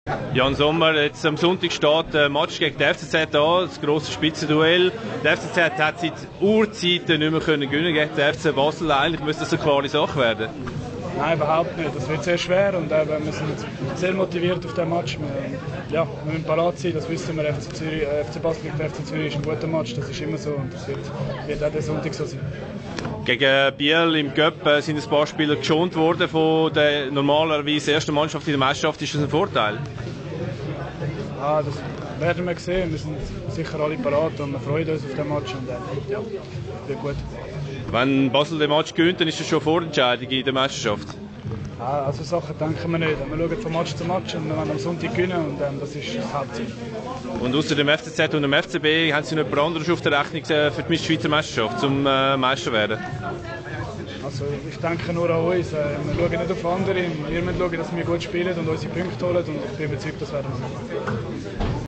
Sommer: "Freuen uns auf Spitzenkampf"
Yann Sommer im Gespräch mit sport.ch